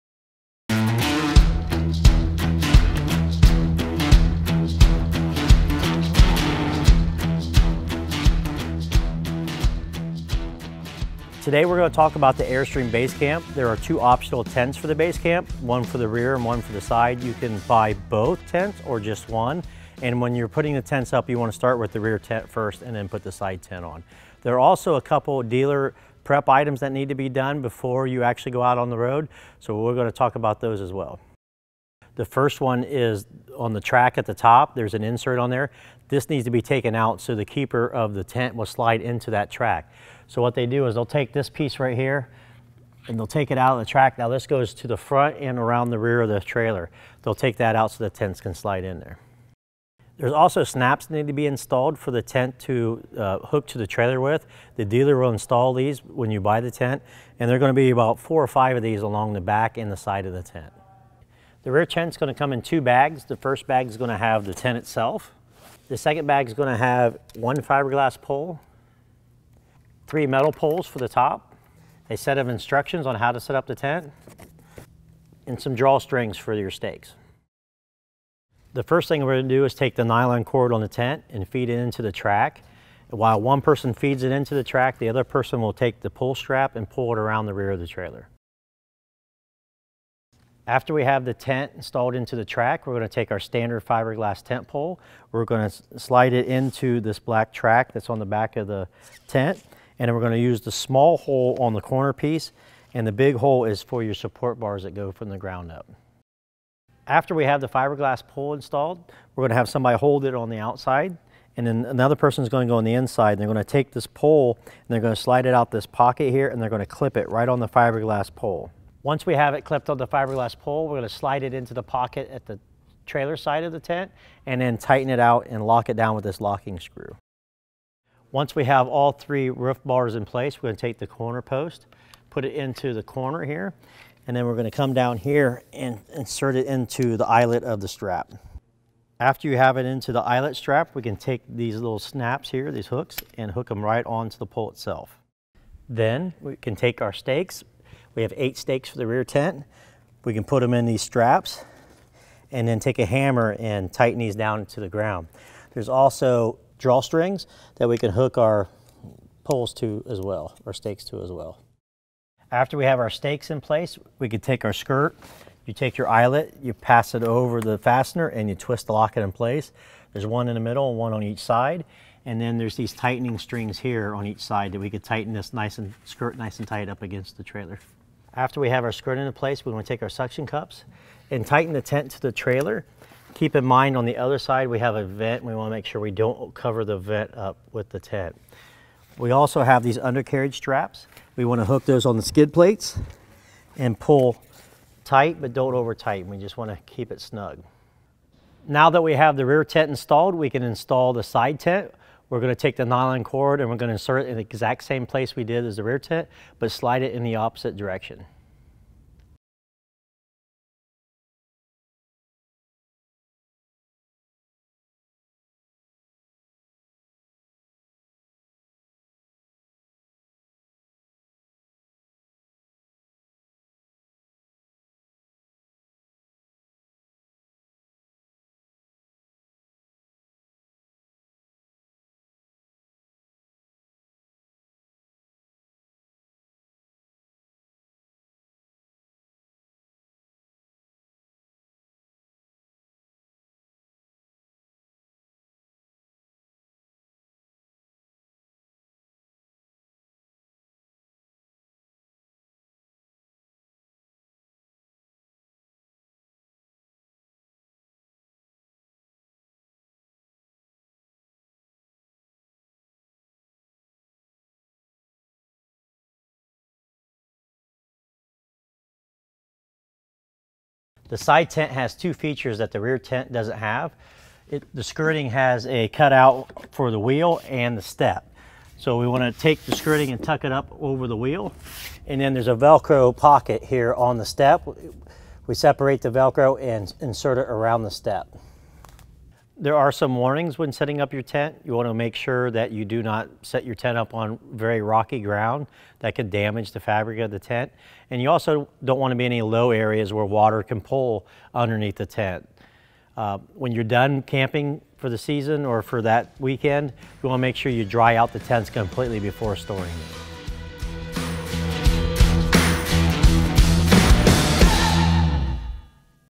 How-To | Setting Up Tents on an Airstream Basecamp® Travel Trailer
Get step-by-step directions for setting up the rear and side tents on the Airstream Basecamp Travel Trailer. Learn more about the Basecamp at airstreambasecamp.com